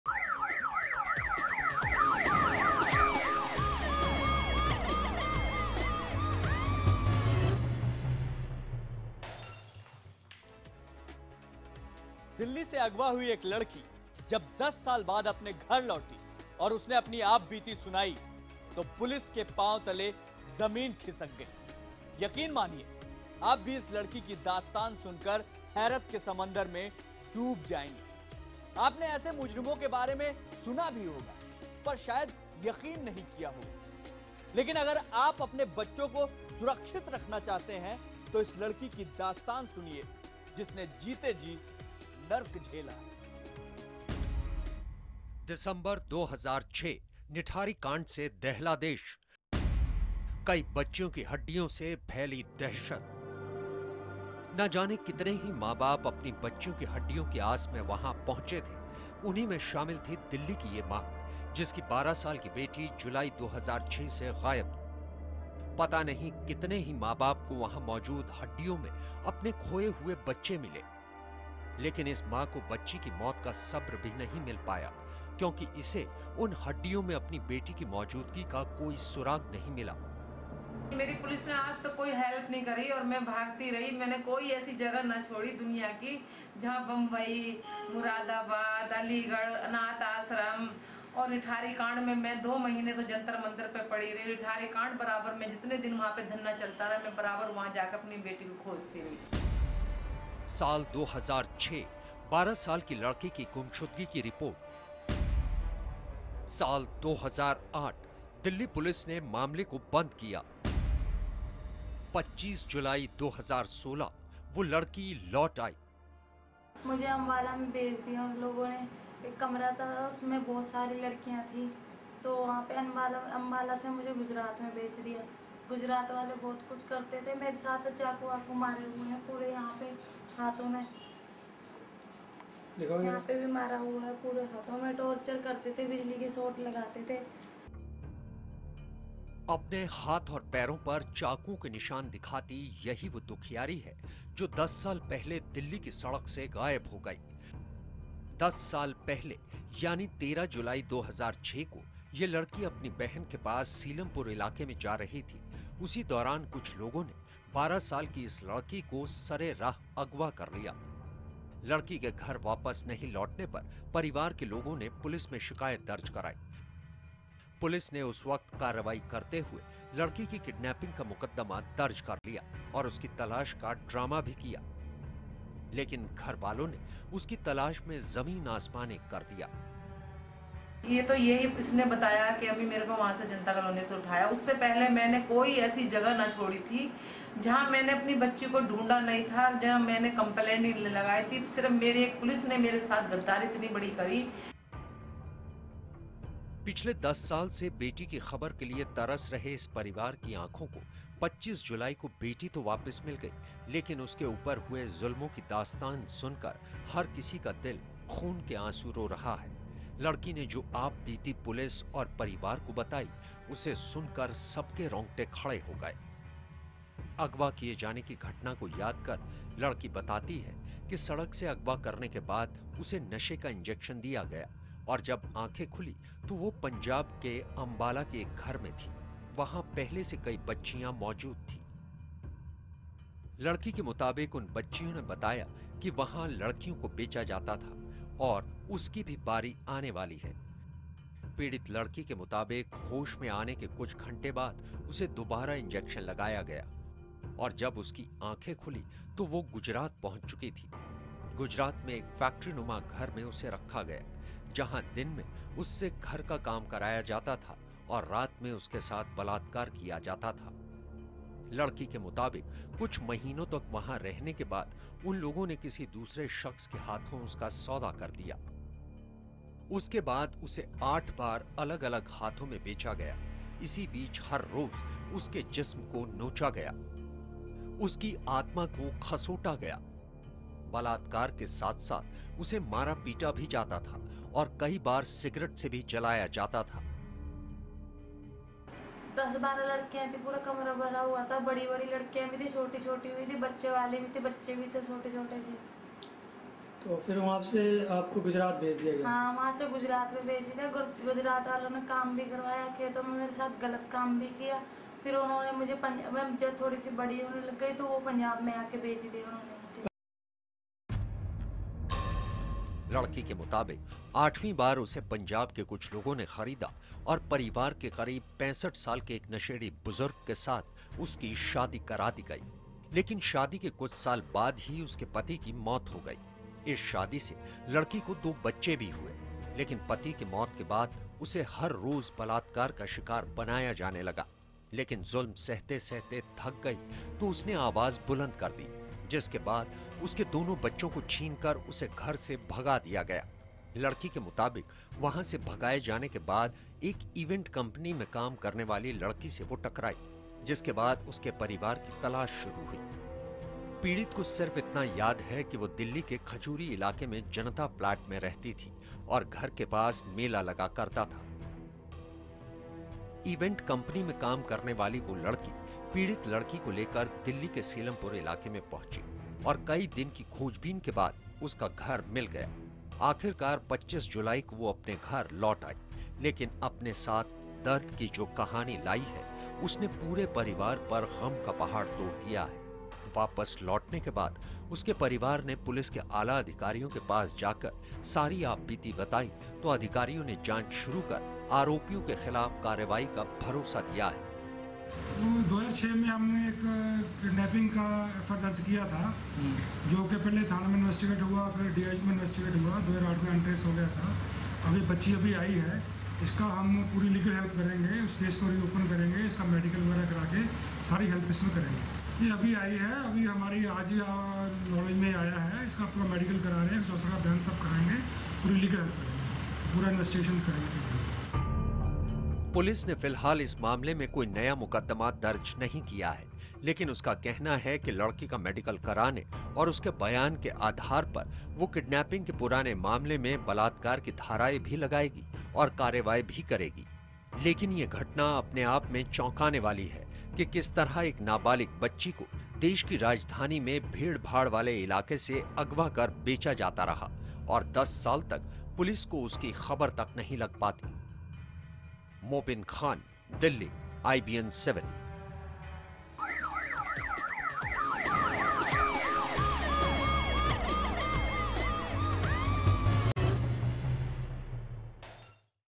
Delhi Se Agwa Ki Gayi Is Ladki KO 8 Baar Becha Gaya, 10 Saal Tak Hota Raha Rape
दिल्ली से 10  साल पहले अगवा हुई एक लड़की जब घर लौटकर अपनी आप बीती सुनाई तो पुलिस के पांव तले से जमीन खिसक गई। 10 साल पहले अगवा हुई इस लड़की को खोजने के लिए मां-बाप ने जमीं आसमान एक कर दिया था लेकिन लड़की का कुछ पता नहीं चला।